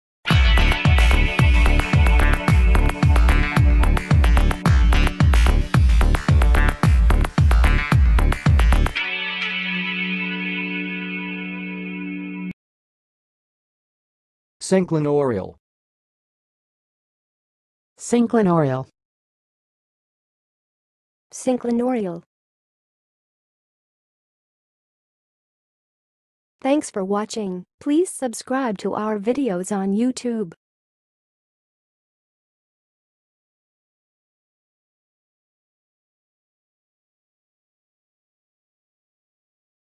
How to say "synclinorial"! (High Quality Voices)
Watch in this video how to say and pronounce "synclinorial"!